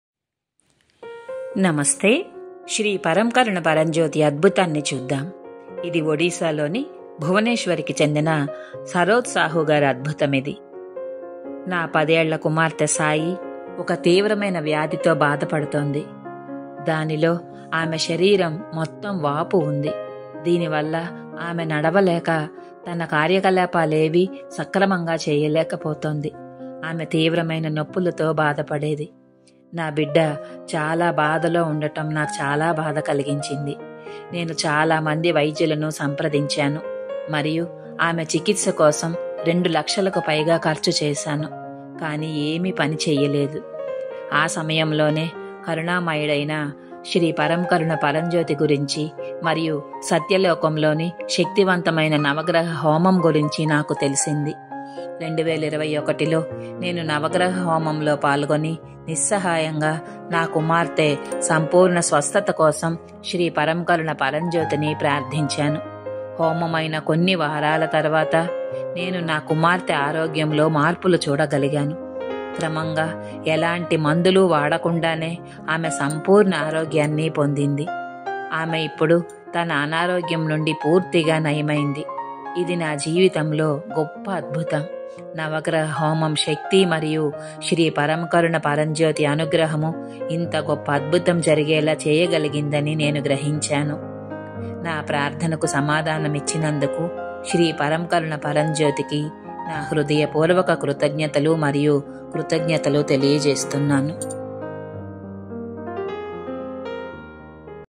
Telugu 03/09. After Navagraha havan she regained her original health without any medication.
My 10-year-old daughter Sai was suffering from a critical disease in which there was swelling in her whole body. It was very painful for me to see my child in so much pain. I consulted many doctors and spent more than 2 lakhs on her treatment, but nothing worked. After a few weeks of havan, I could she changes in my daughter's health. She is now completely cured of her illness. I express gratitude from the core of my heart and loads of thanks to Sri Paramkaruna Paramjyoti for answering my prayer.